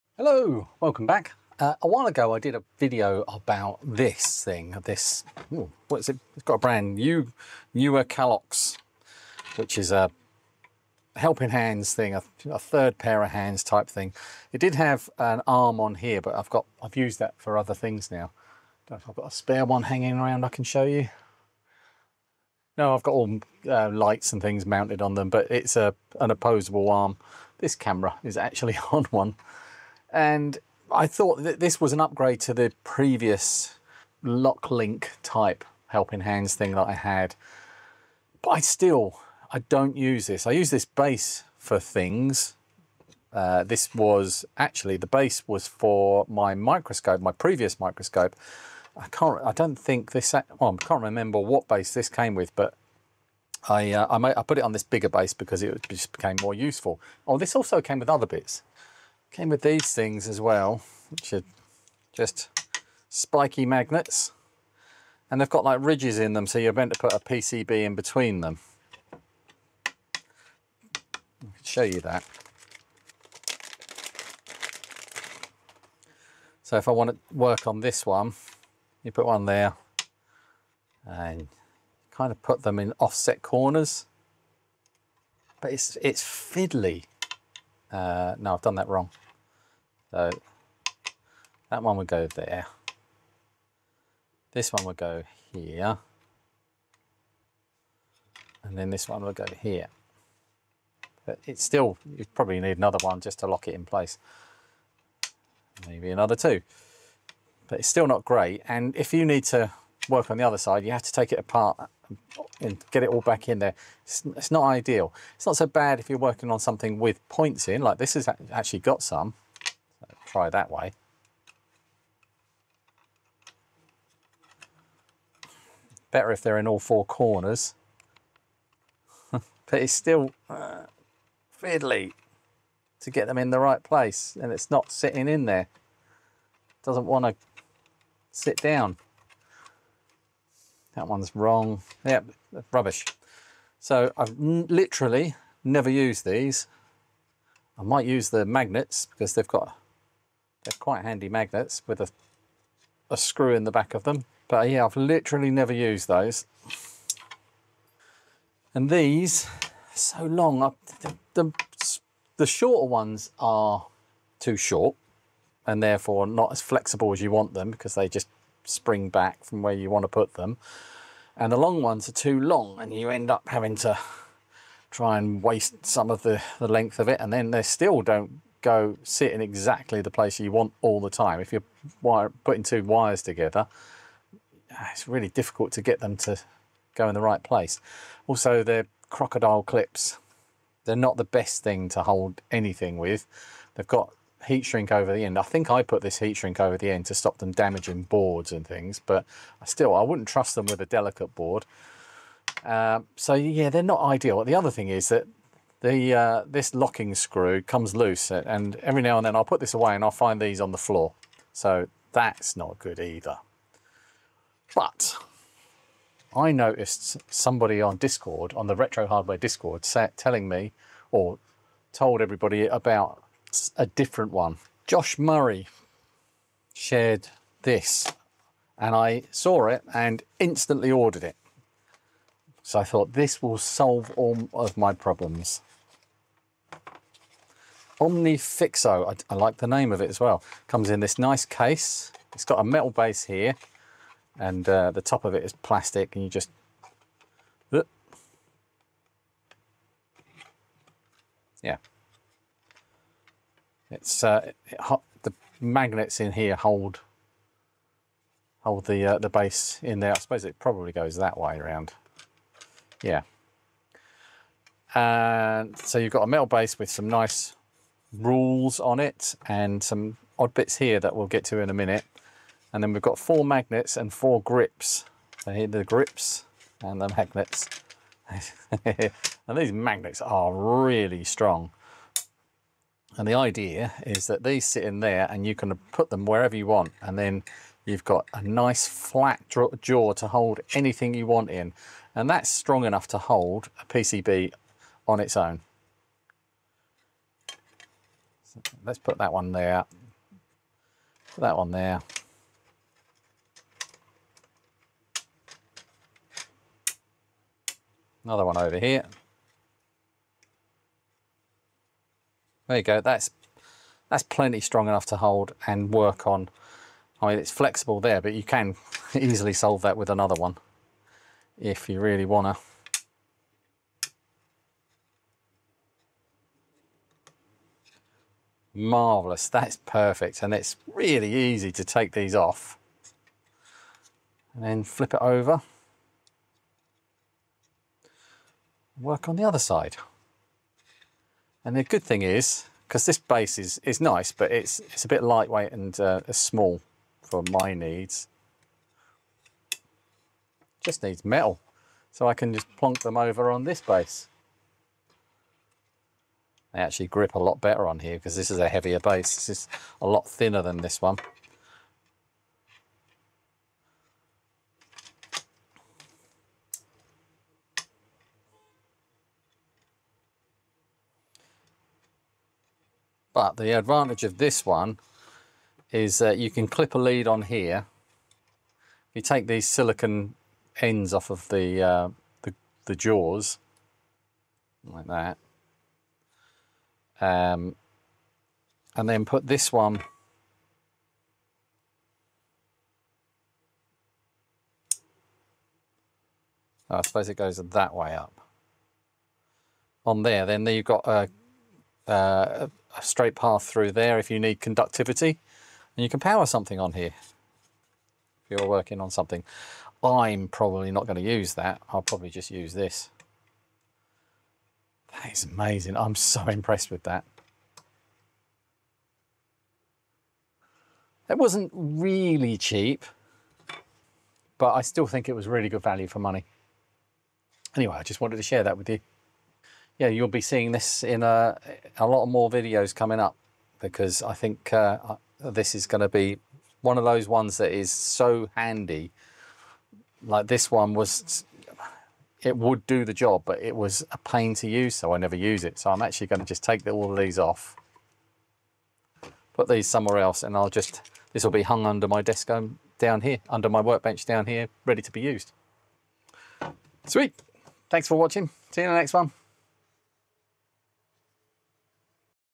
Omnifixo - More Hands for your Workbench
In this video I review the Omnifixo PCB holder, a modular helping hands tool designed for electronics and soldering. I show how the magnetic clamps and flexible arms work, how it handles holding circuit boards, and share my thoughts on whether it’s a better option than traditional helping hands or PCB vices.

MAIN CHANNEL!

Amazon Wishlist